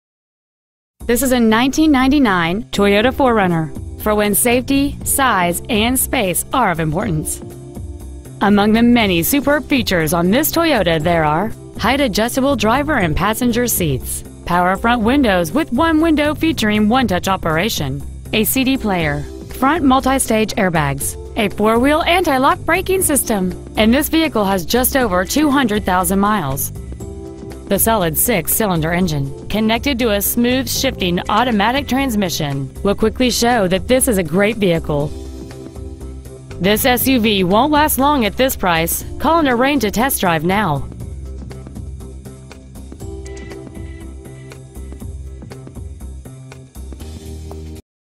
1999 Toyota 4Runner Tulsa OK
Year: 1999
 Make: Toyota
 Model: 4Runner
 Engine: 3.4L V-6cyl
 Trans.: Automatic
 Exterior: Green
 Miles: 203,867
 Interior: Gray

 6868 East B.A. Frontage Rd.

 Preowned 1999 Toyota 4Runner Tulsa OK